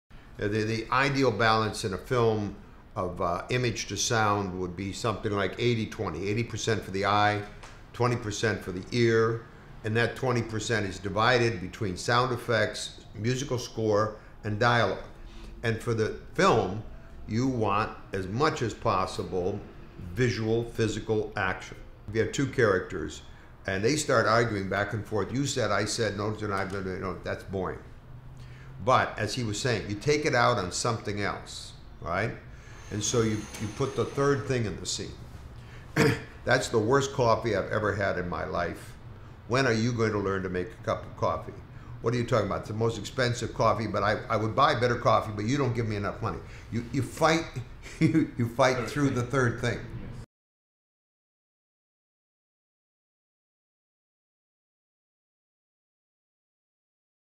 Robert McKee x TV SERIES FESTVAL
A snippet of our conversation with Robert McKee
Berlin TV SERIES FESTVAL 2018